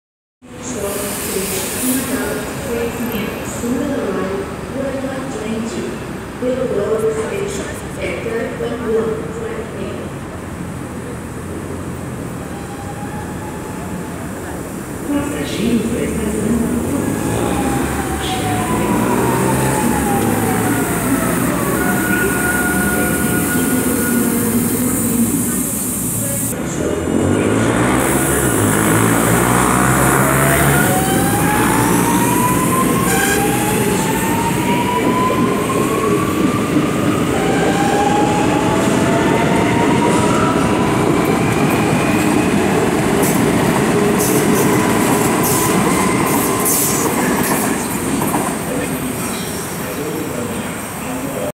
Trem Alstom chegando e saindo de São Cristóvão (2)
Gravado no dia 9 de Março de 2018.